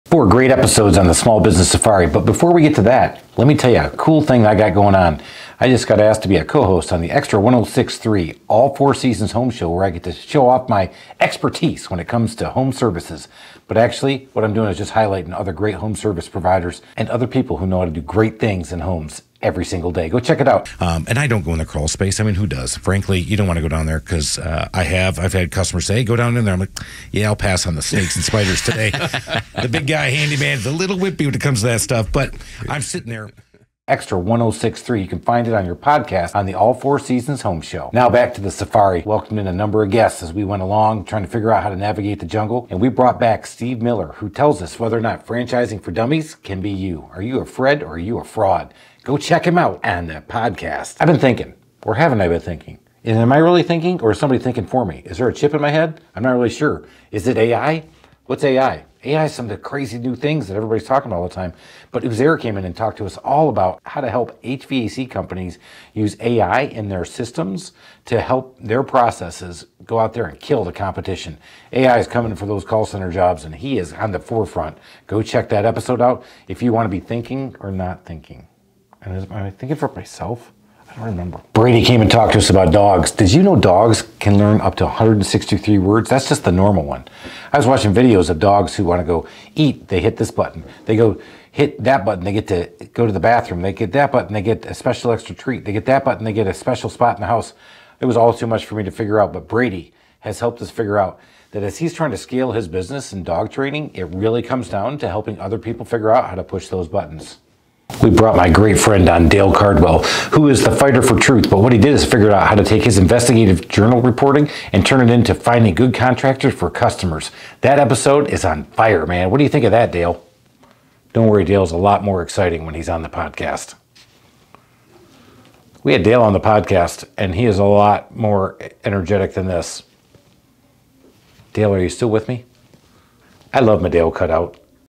Dale Cardwell Makes it to the Podcast & I'm a New Radio Station Co-Host !
A ton of great guests this month on the Small Business Safari Podcast! Dale Cardwell, Steve Miller, Brady Foulk, and Uzair Ahmed. Also, I'm offically a co-host on Xtra's 106.3 All Four Seasons Home Show!